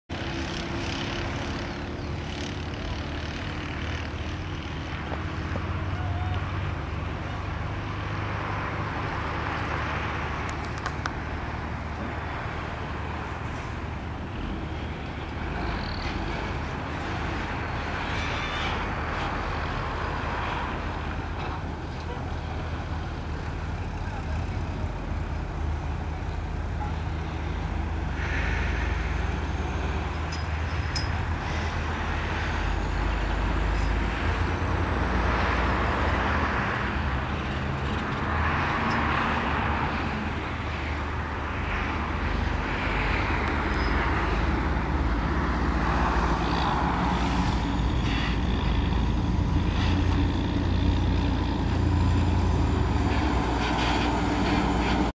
Manguito, colocacion de prelosas.
Libramiento sur, Tapachula Chiapas. Colocacón de prelosas.